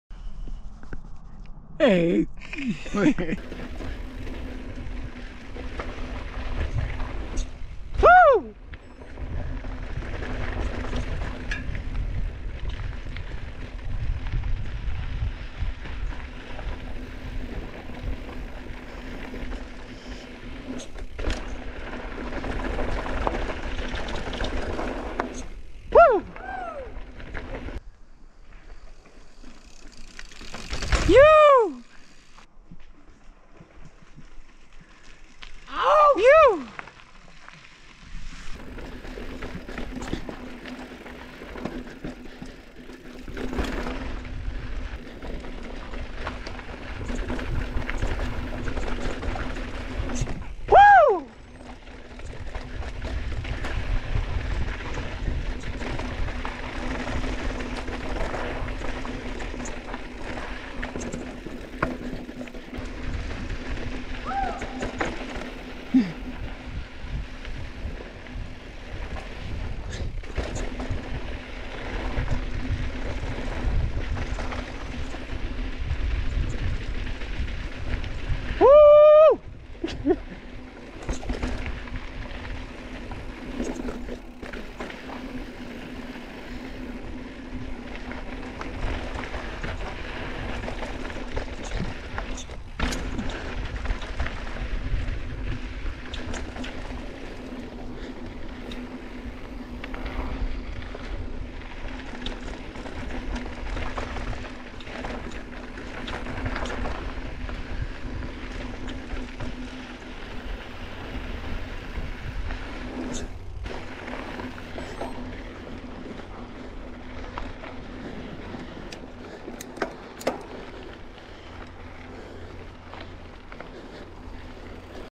Truckee Mountain Biking Clips
Just playing with 360 a little and hitting some jumps. Flow state.